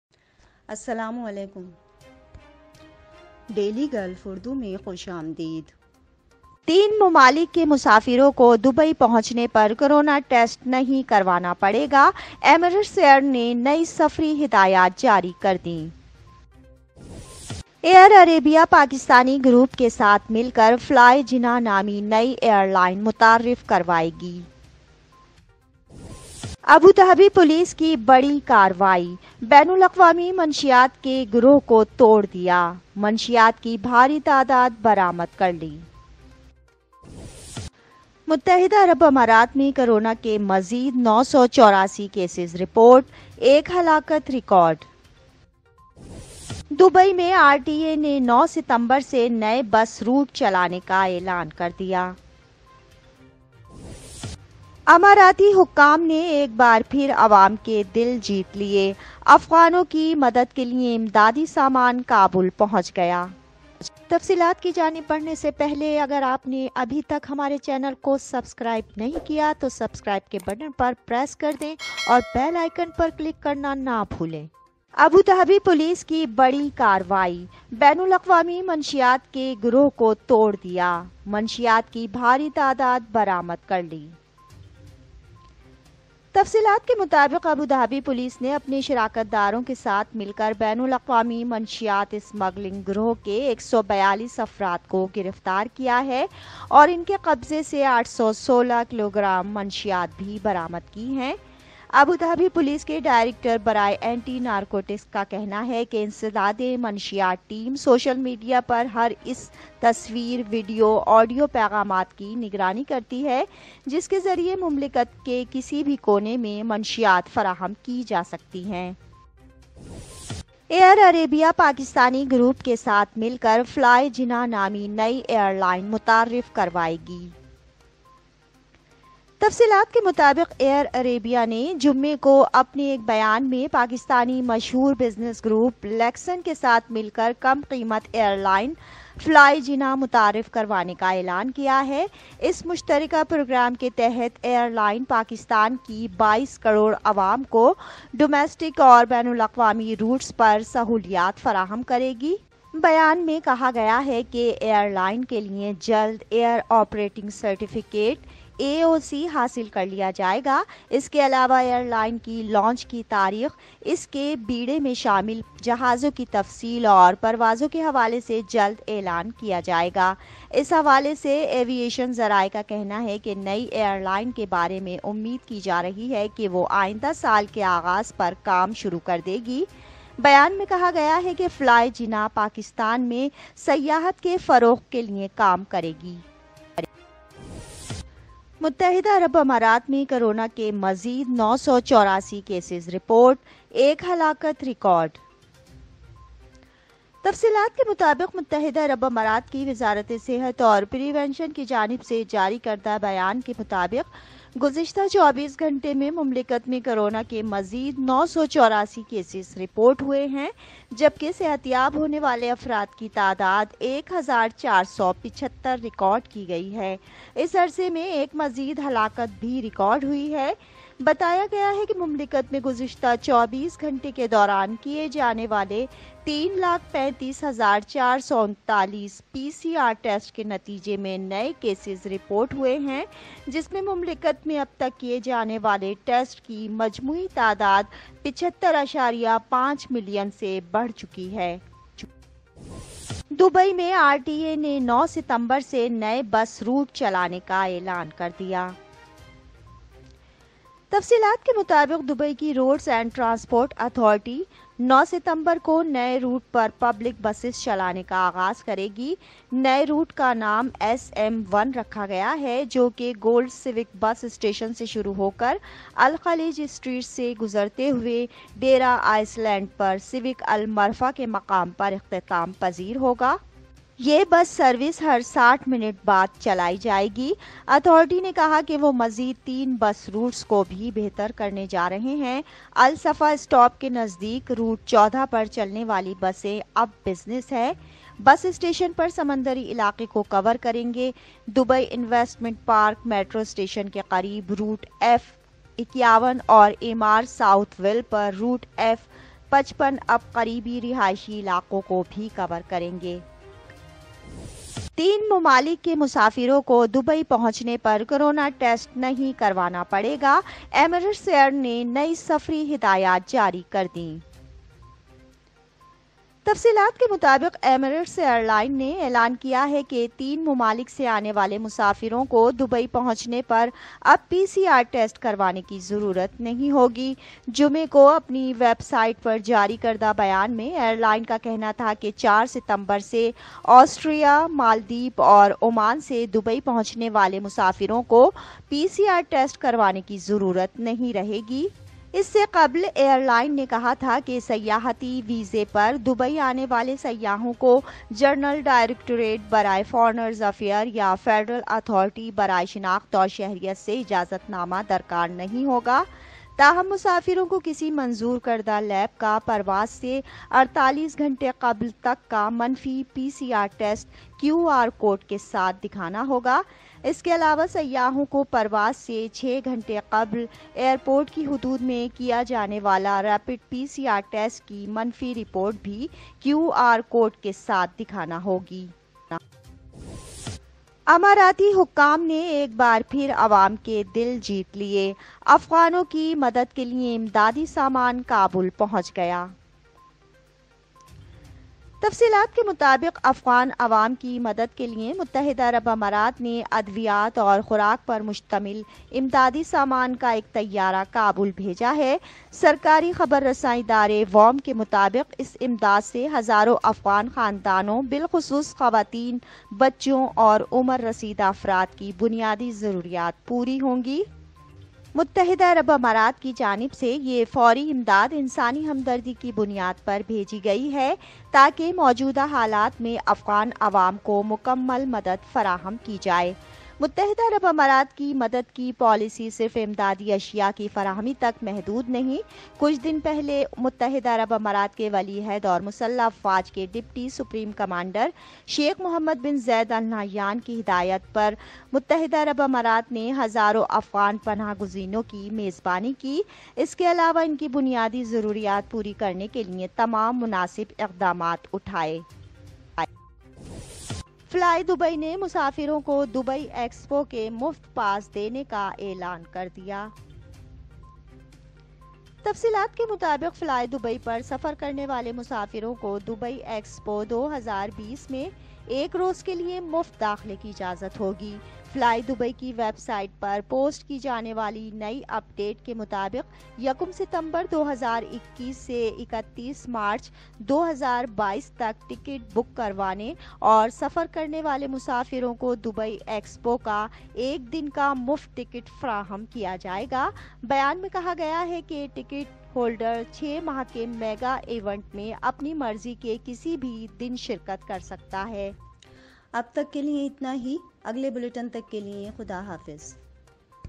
Gulf news | Air Arabia deal with Pakistan | Abu dhabi police arrested drugs gang | Dubai New Bus_
Air arabia deal with Pakistan | Abu dhabi police arrested drugs gang | Dubai New Bus station